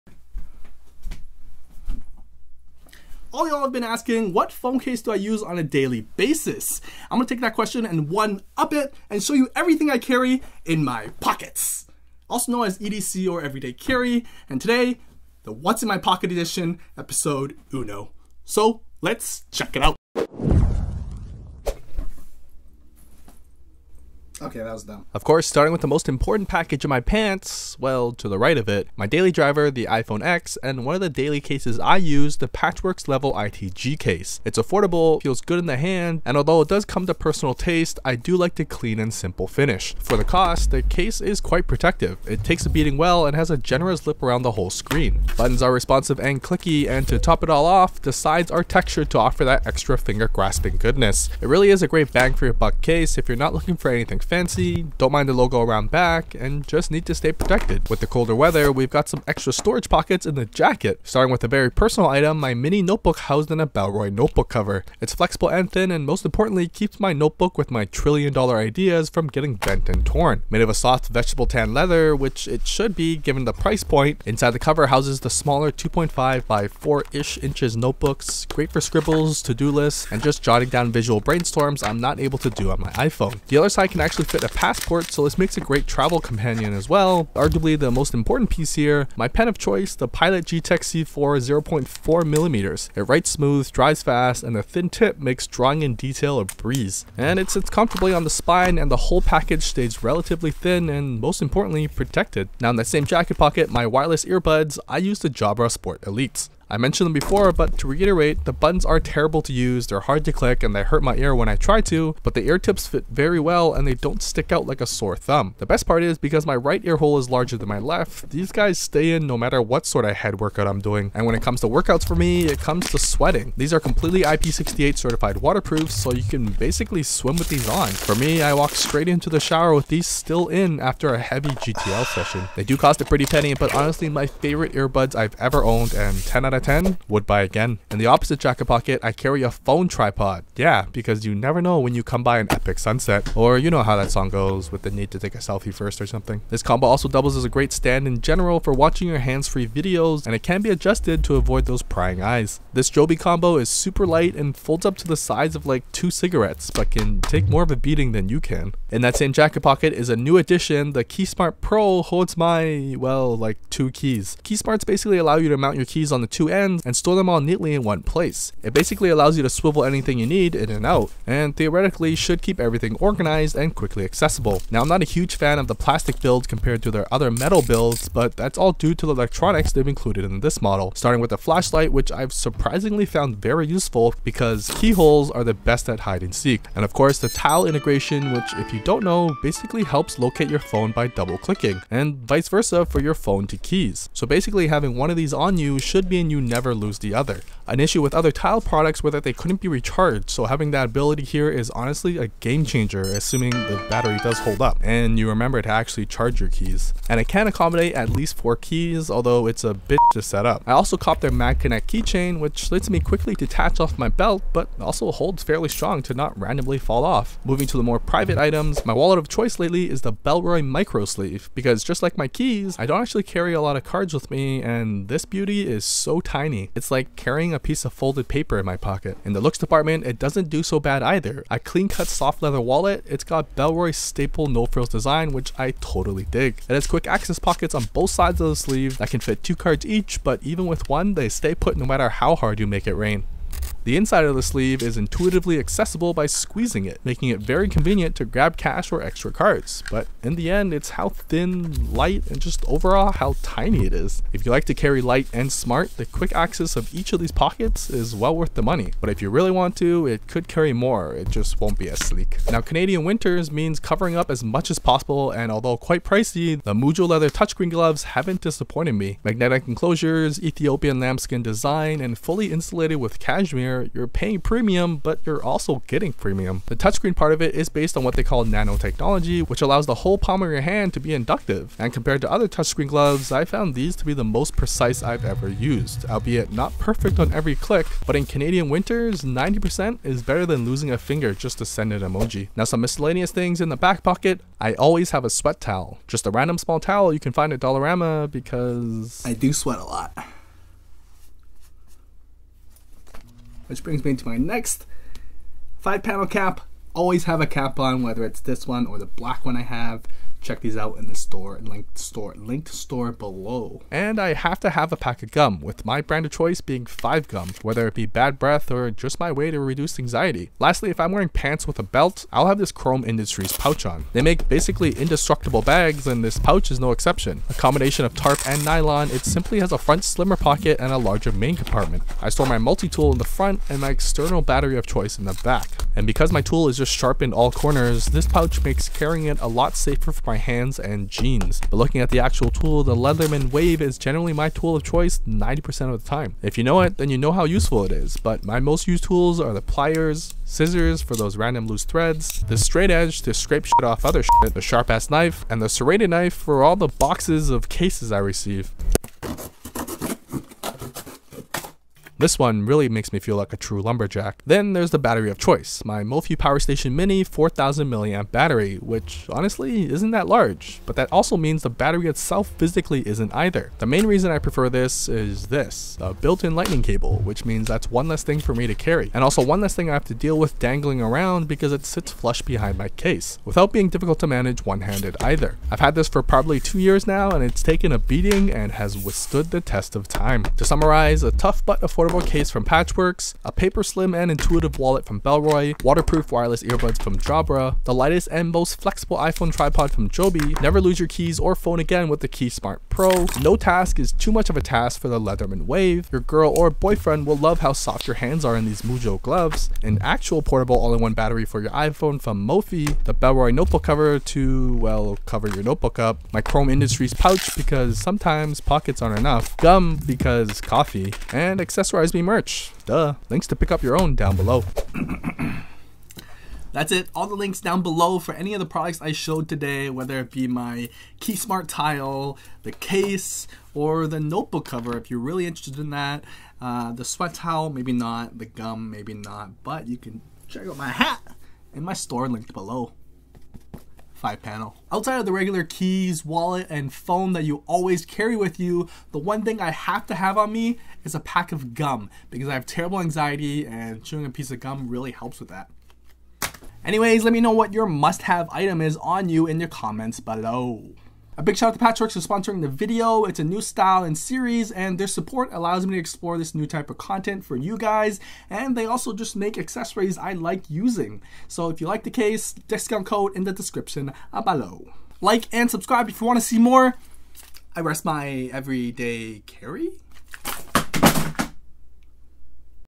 What's In My Pockets Ep. 1 - EDC (Everyday Carry) of April 2018
I'm working on some new series and transitioning my content into more Everyday Carry style videos, starting with a new monthly series covering What's In My Pockets. I provide a pocket dump of the items I carry with me during this cold Canadian winter month and hopefully you discover something new to add to your pockets!

FREE AMAZON PRIME TRIAL